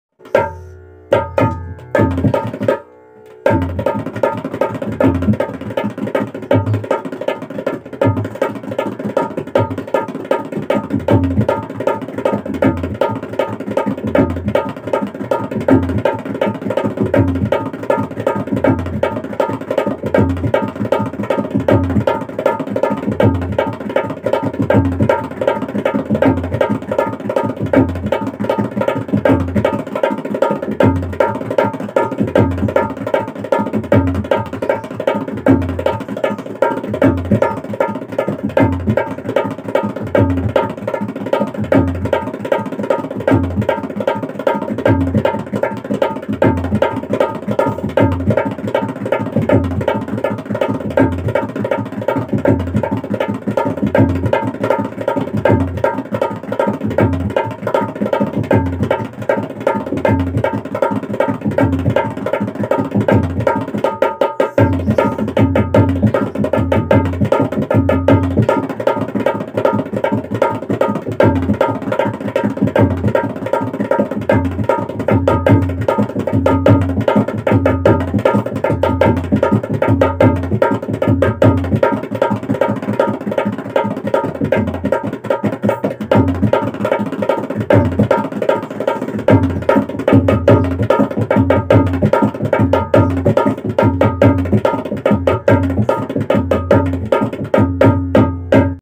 Dholak पर रेस में चलने वाले घोड़े (horses) की आवाज | tirekit | play by Yog Raj Rana
Dholak पर रेस में चलने वाले घोड़े (horses) की आवाज  | play by Yog Raj Rana (HimalayanYogi)